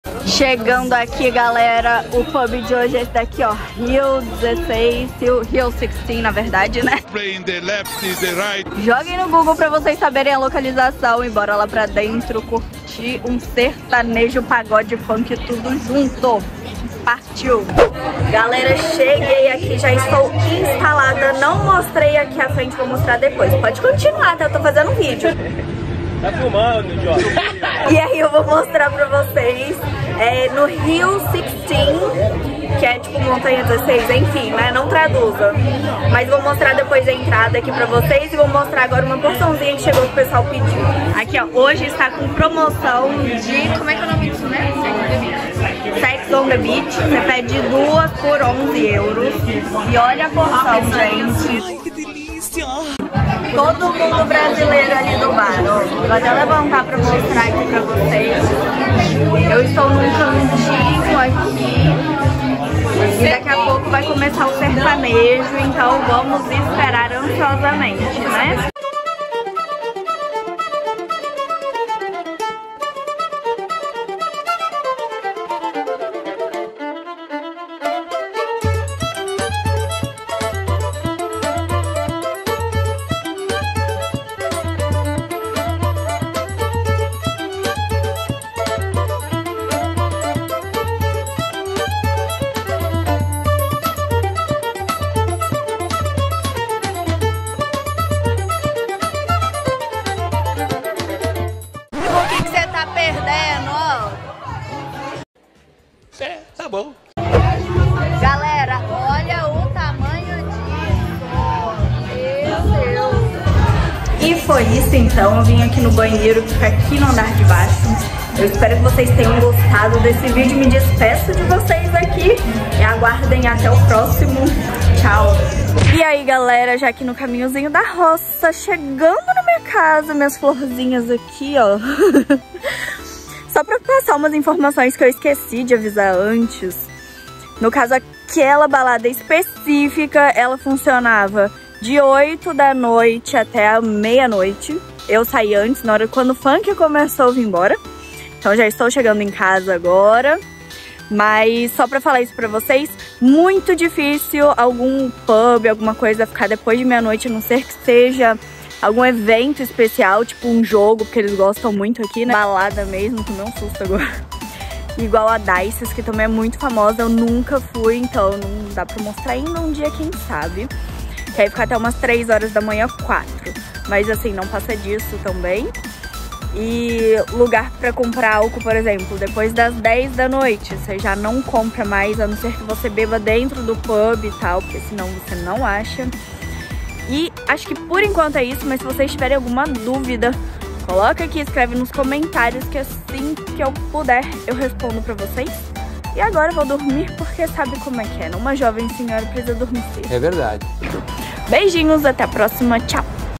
Um dia de musica Brasileira em um Pub na Irlanda
Ja vou deixar aqui para vocês a localização desse Pub Hill 16

Procurem o insta deles que tem a programação, sempre tem alguma coisa legal, gostei bastante do ambiente acho que vale a pena uma visitinha. Já dou a dica que o ponto do ônibus 41 fica a uma quadra de lá!